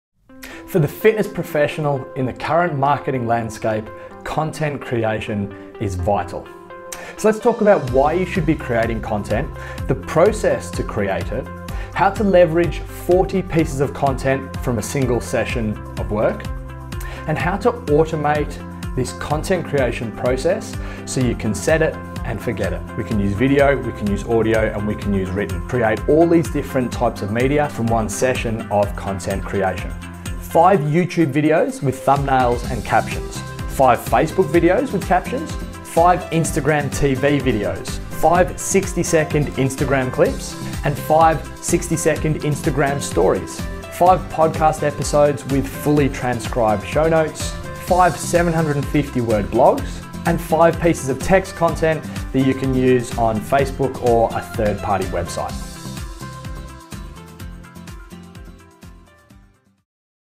TEASER: Content Creation MASTERCLASS for Fitness Pros
This comprehensive 35 minute video masterclass will teach you everything you need to know about creating content to develop your brand, increase your exposure, and acquire new clients.